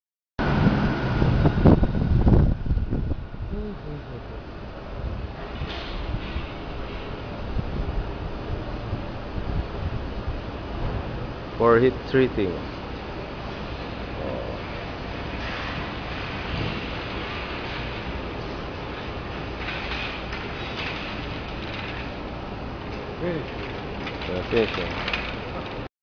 Panzer Bike Factory Tour
Factory can built all kind of frame and Aluminum Heat Treating